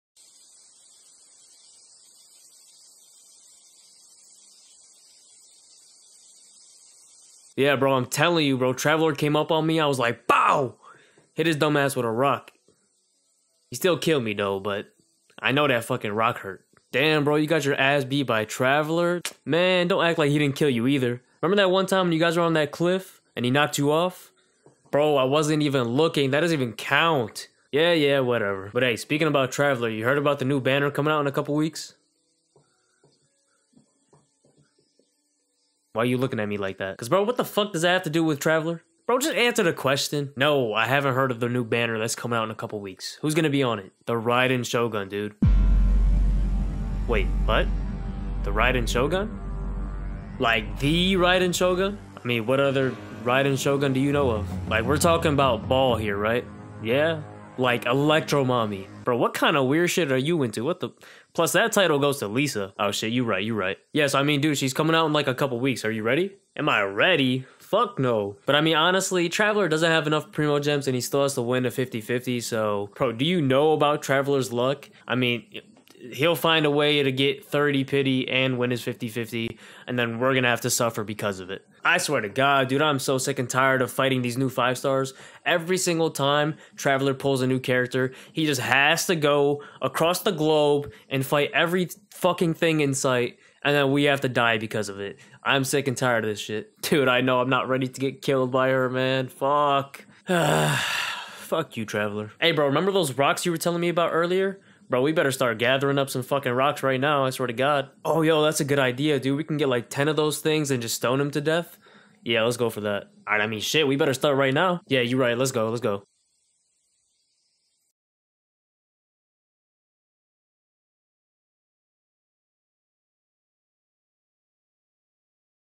If hilichurls could talk...
Just a couple of hilichurls giving their thoughts about the upcoming banner featuring baal, aka the raiden shogun.